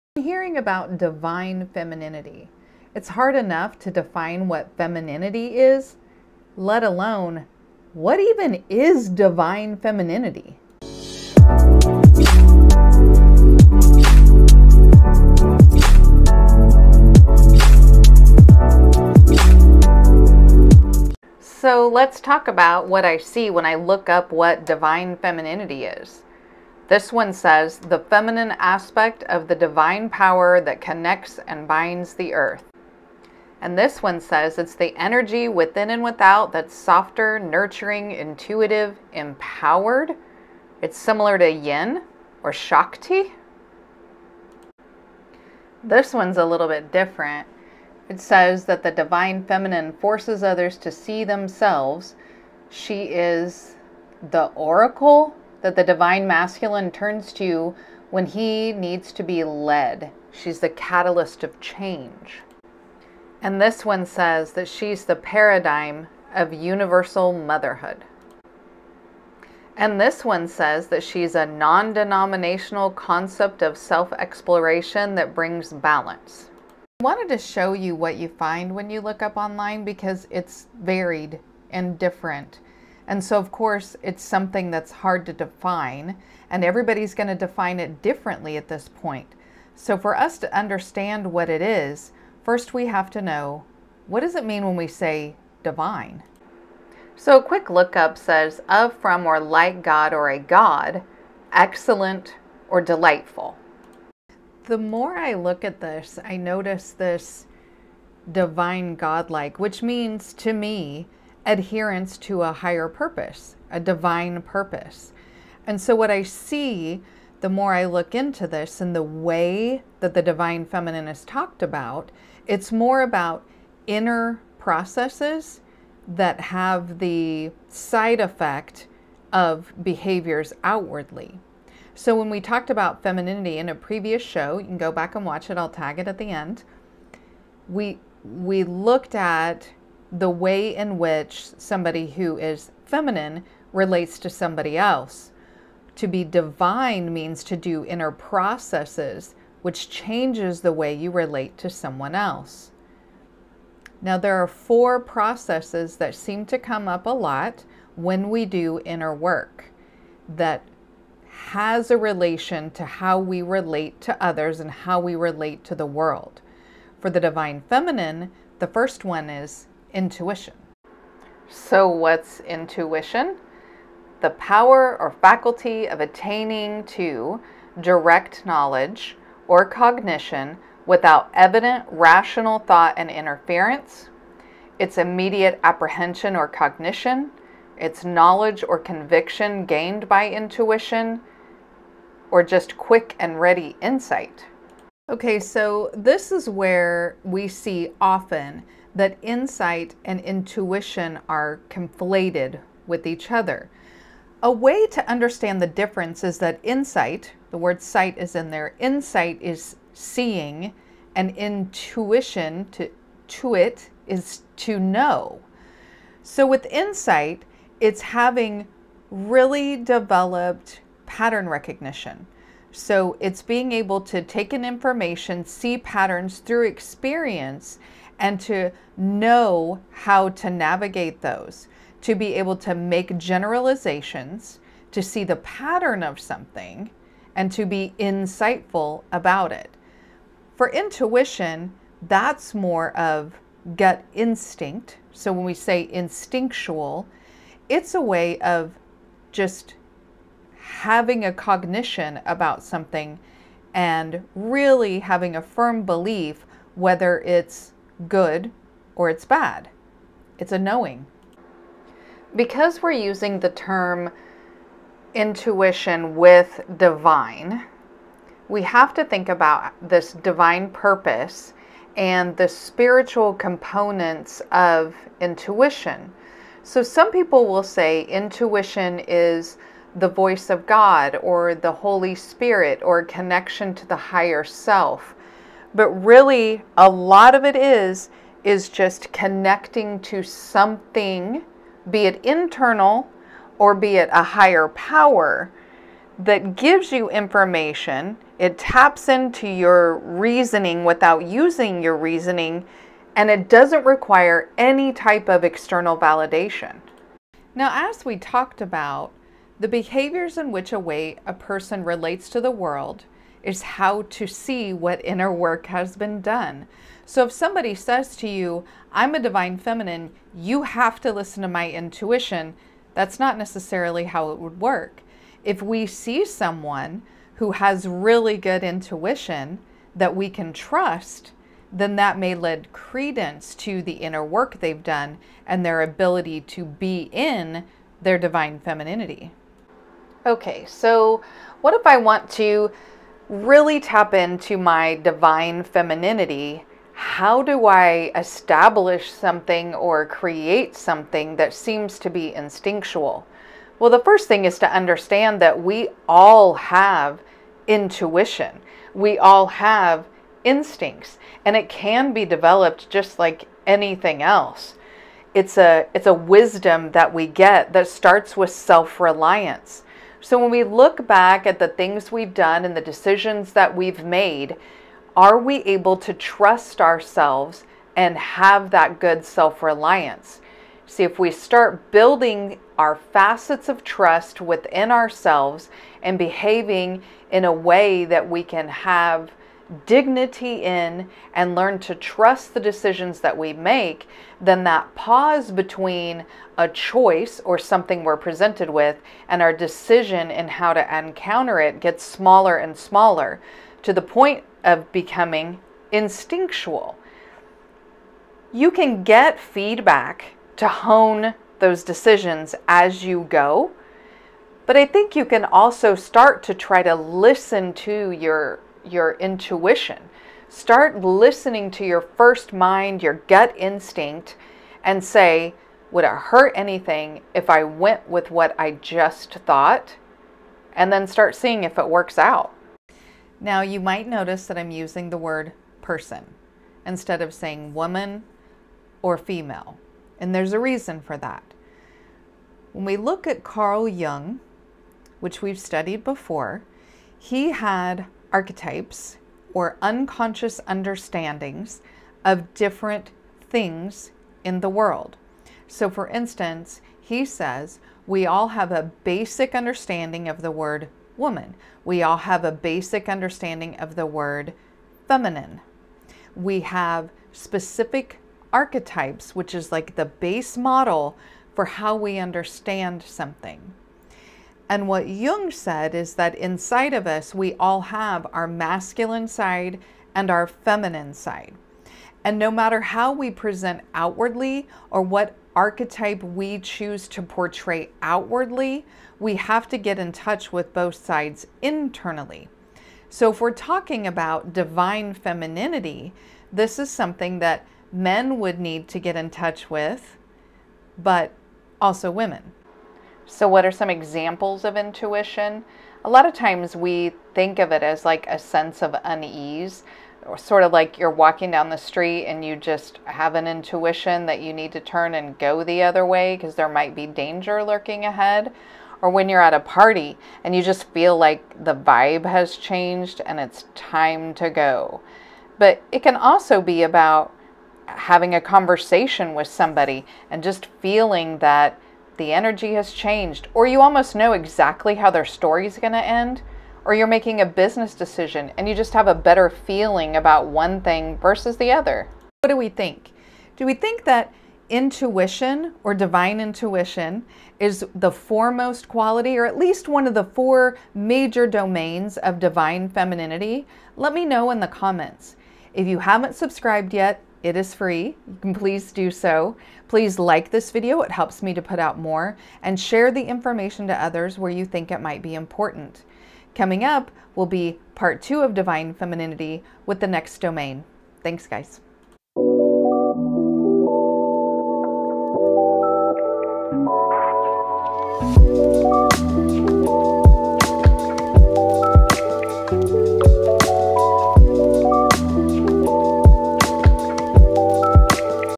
Divine Femininity Part One: Intuition
It's so difficult to define femininity, let alone DIVINE femininity.  Join me as we start the journey to define divine feminity in this four part series, starting with Intuition.